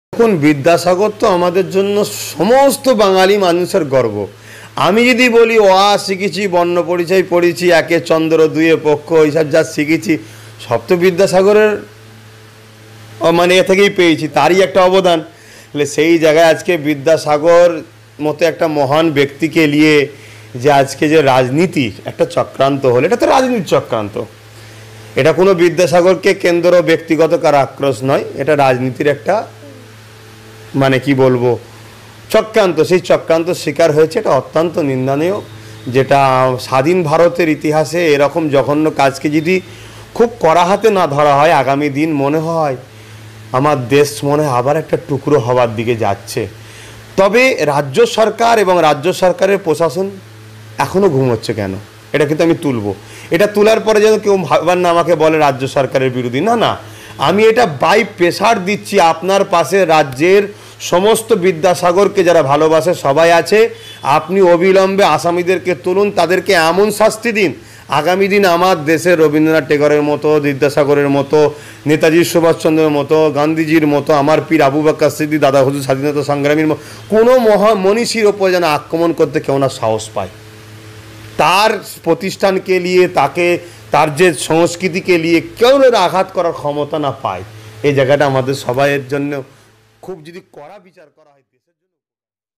বিদ্যাসাগর এর মূর্তি ভাঙা নিয়ে সরকারকে কড়া বার্তা পীরজাদা আব্বাস সিদ্দিকীর
R.M. Media Bagnan